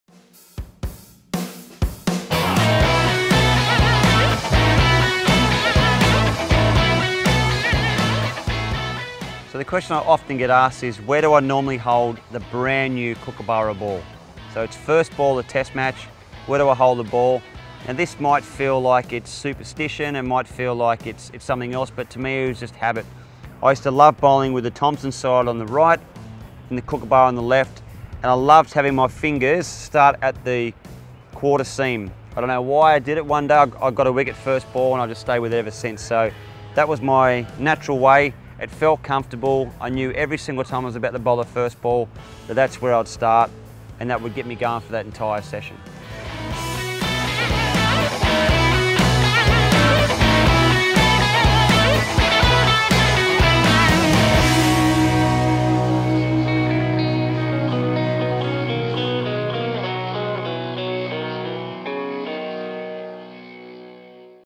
HOW TO BOWL WITH A NEW KOOKABURRA BALL I BRETT LEE TV I MASTERCLASS
Bowling with a new Kookaburra ball needs a specific skill set!